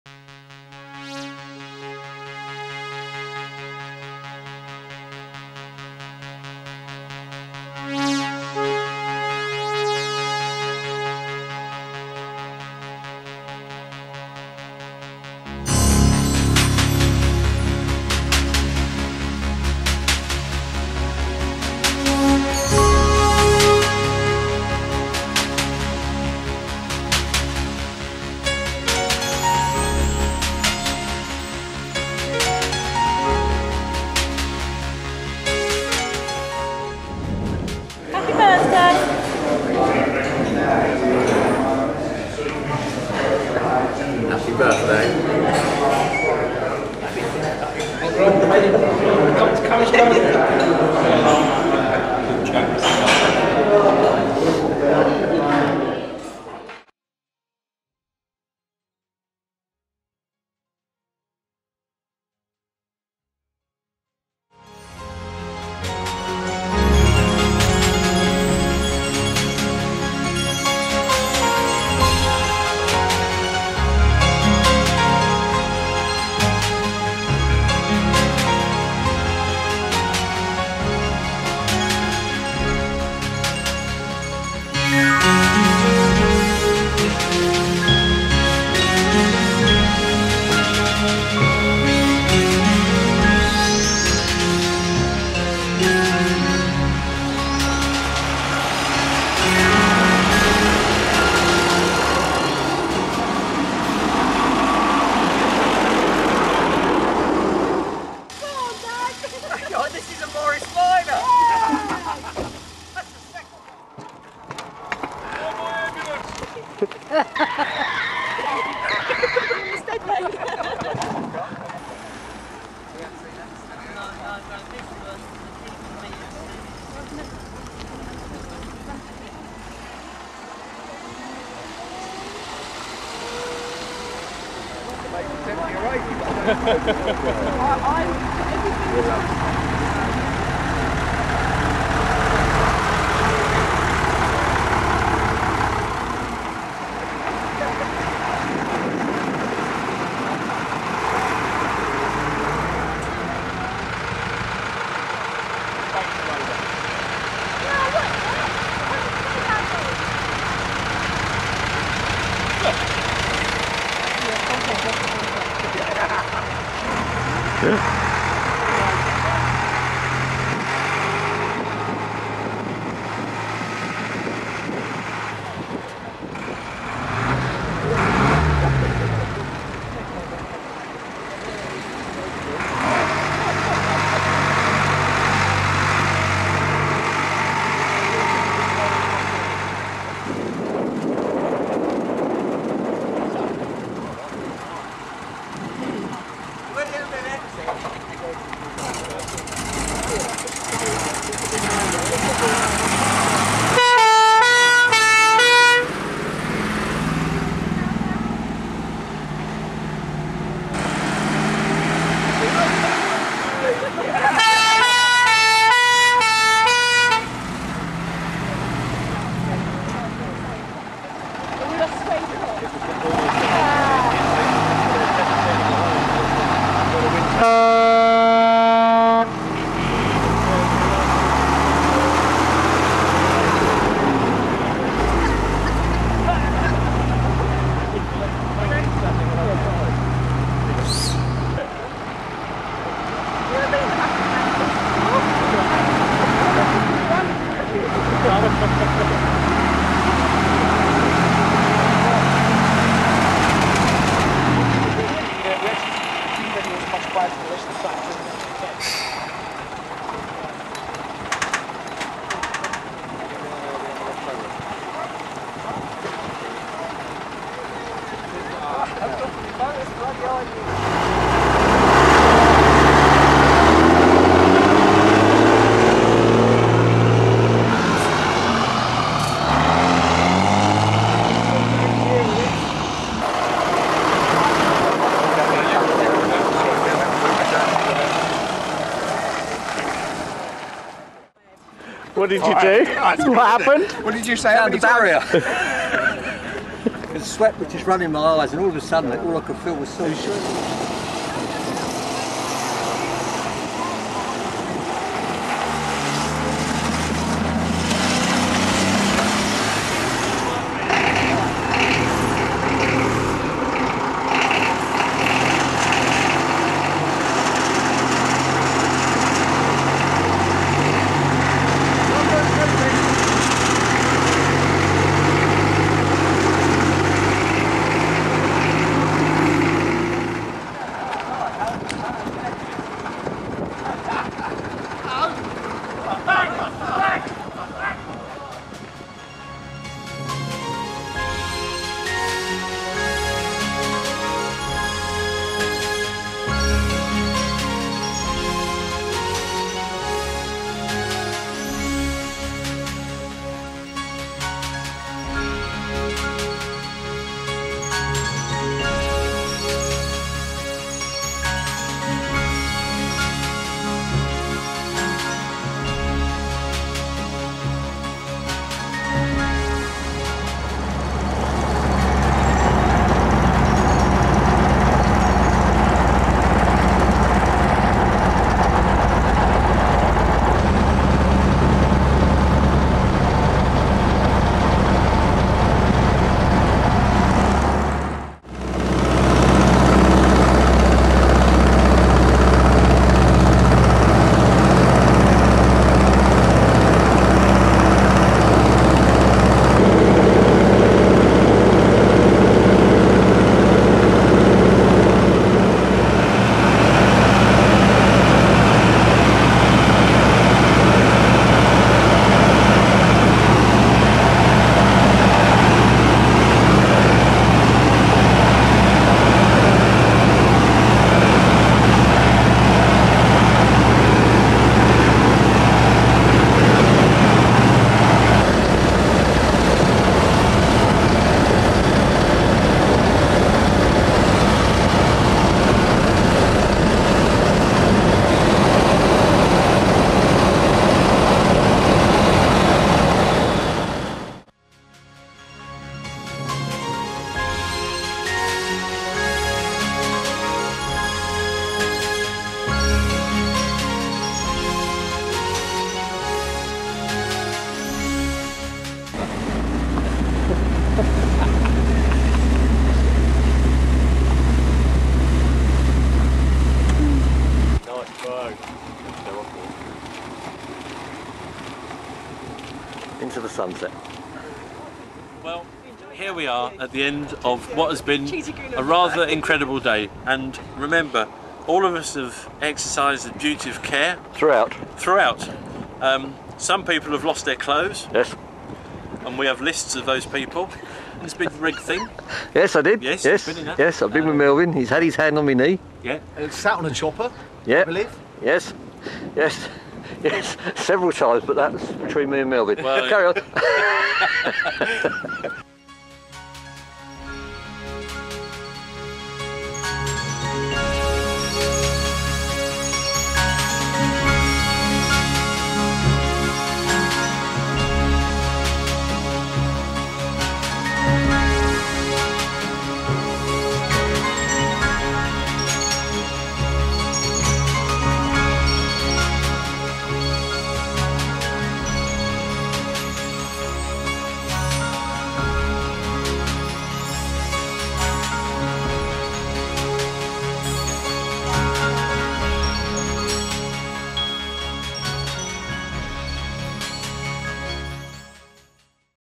Andy's 60th Birthday surprise, or how the 'b'steads set me up!
A group of my friends, relatives and my wife, treated me to a very different sort of 60th birthday surprise. - I awoke thinking  I was going to have a peaceful day attending a meeting in Surrey, followed by a peaceful evening studying the 'dancing skills' of some young ladies at the Heathrow Spearmint Rhino.

Instead they had been plotting and organising for months an incredible treat, in which I was given the chance to drive (or in some case ride in) a selection of some sixty special vehicles. These ranged from a Skate Board, to the Brooklands Concord. It was a most wonderful and memorable day, made the more enjoyable because of the all effort they had gone to.